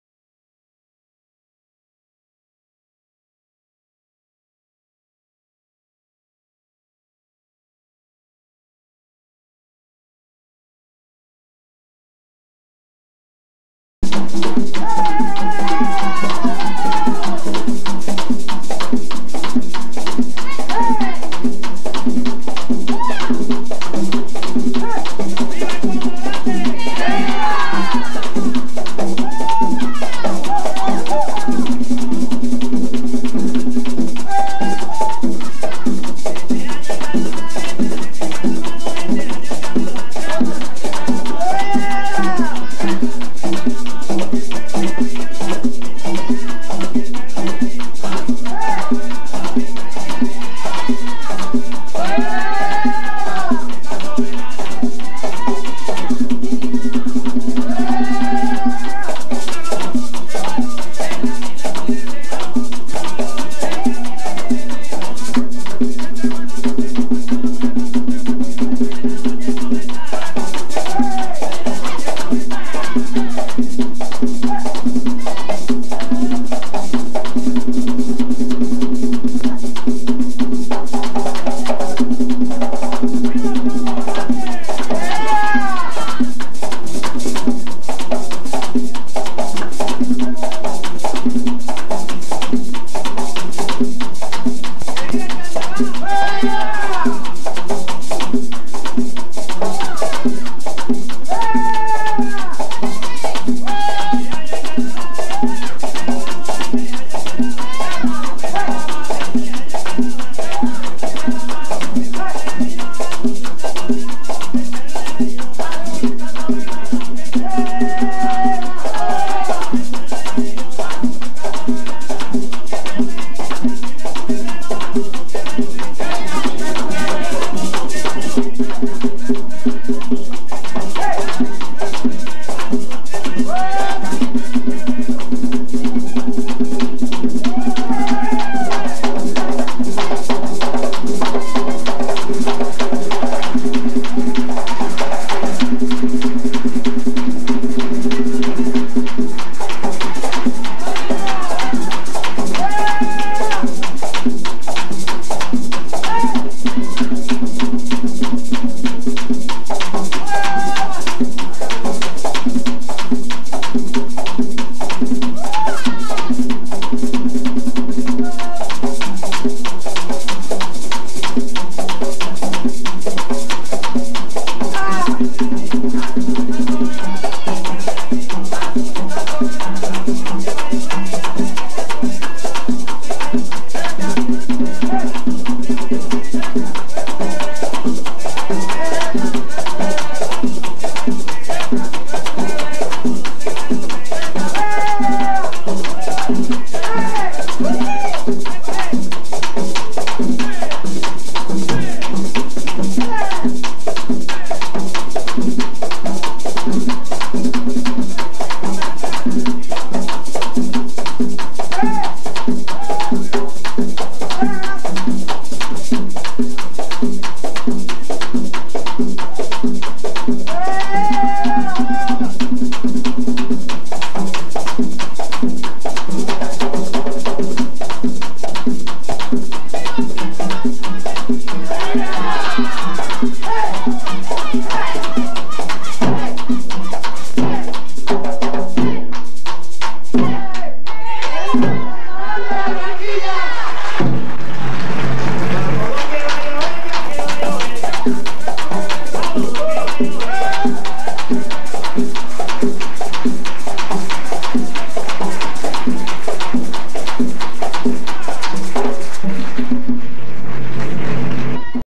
Danzas Folclóricas de la Universidad Piloto de Colombia, I parte, Bogotá, 2002
Imágenes de la presentación artística y cultural del grupo de danzas Folclóricas de la Universidad Piloto de Colombia,  evento realizado en el teatro de dicha institución educativa en el año 2002 en la ciudad de Bogotá.  Participaron como músicos entre otros, Gustavo Adolfo Angulo y Manuel Antonio Rodríguez, la comunidad Piloto (Estudiantes, Egresados, Docentes y Administrativos). Este tipo de actividades contribuyen a estrechan los lazos y sentar las bases para soñar proyectos en conjunto con los grupos culturales institucionales, permitiendo que toda la comunidad universitaria disfrute su paso por las instituciones y realmente viva la vida universitaria.